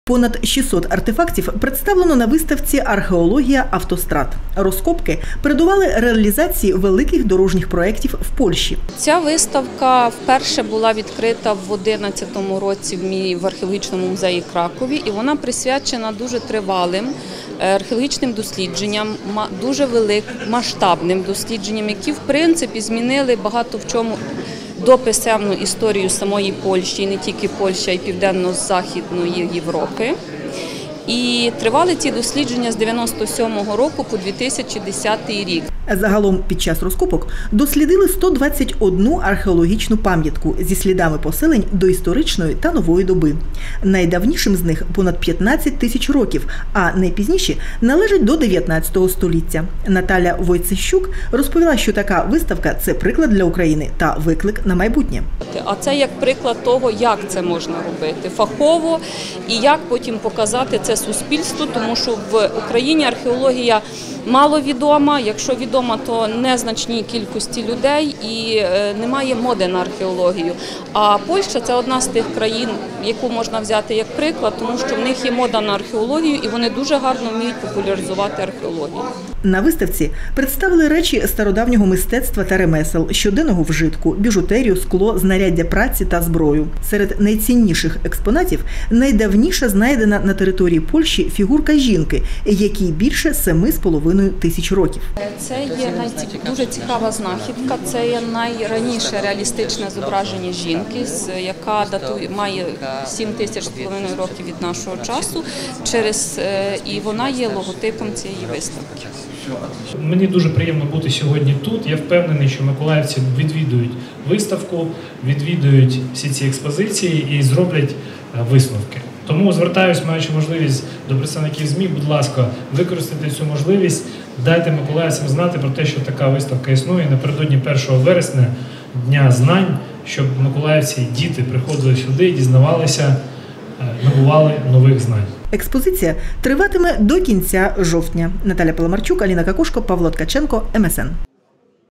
Виставка в музеї суднобудування та флоту «Археологія автострад» 28.08.18
«Археологія автострад» - під такою назвою в музеї суднобудування та флоту відкрили виставку, присвячену археологічним розкопкам, які передували будівництву автострад і реалізації великих дорожніх інвестицій в Польщі.